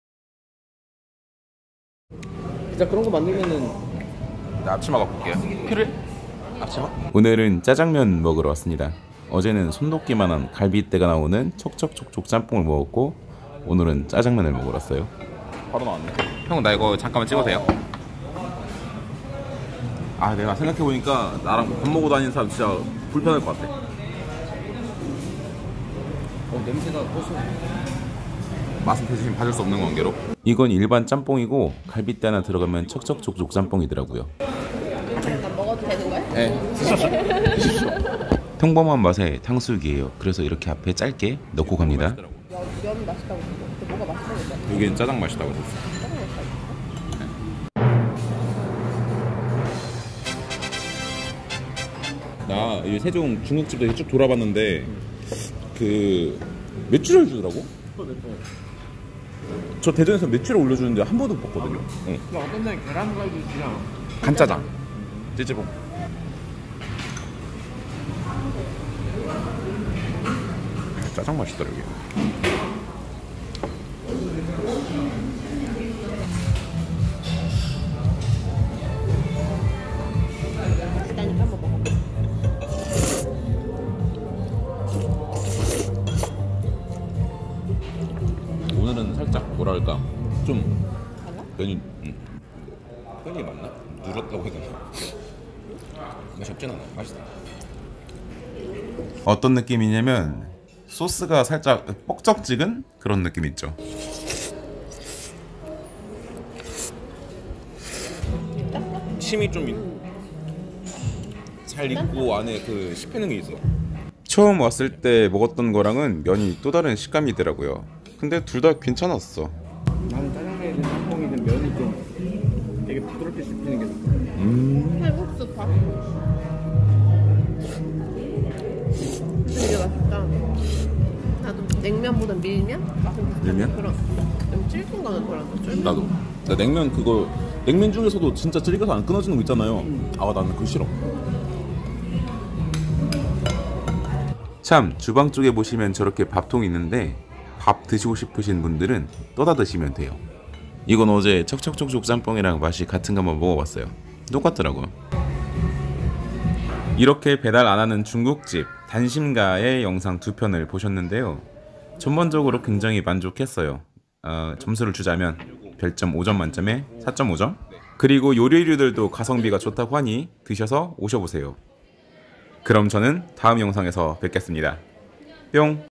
단심가 [2편] 짜장면 | 세종시 맛집 반곡동 | Jjajangmyun
단심가 2편입니다.
이번엔 짜장면을 먹어보았는데요
처음 방문했을때의 그 탱글탱글하고 쫀득한 면발이 아닌
조금 덜삶은 느낌의 면심지가 남아있는듯한 식감으로 먹어봤습니다
파스타면의 익힘정도로 표현하자면 알덴테보다 조금 더 익은정도?
먹는데에 문제는 전혀 없었고 오히려 새로운 식감이었어요
맛있게 먹었고
결론은 단심가는 척척쪽쪽짬뽕이 짱이다!
(짜장도 한번드셔보시길 바라구요)
그리고 요리류들도 가성비가 좋다고 합니다
뭐 이정도면 제가 아는 선에서 설명은 거의 끝난것 같아요

하지만 여러분들은 안끝난거 아시죠
아.시.겠.어.요?
ㅋㅋㅋ

다음영상에서 또 봐요
보아주시고 읽어주셔서 감사합니다
뿅

단심가
세종특별자치시 한누리대로 1948 115호
세종특별자치시 반곡동 771-68
평일 11:00 - 23:00 브레이크타임 14:00~17:00
주말 11:00 - 21:00